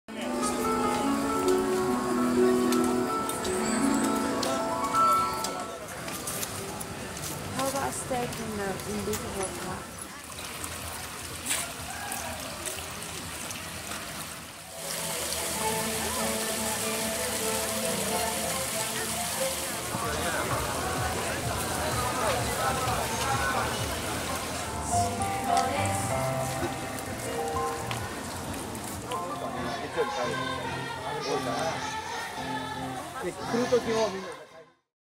ハウステンボスで「ガーデニングW杯」開催
庭づくりの腕を競う「第３回ガーデニングワールドカップフラワーショー」（朝日新聞社など共催）が２９日、長崎県佐世保市のハウステンボスで始まった。世界で活躍する庭園デザイナーが手がけた個性豊かな庭を見ようと、開場と同時に多くのガーデニング愛好家が詰めかけた。１０月８日まで。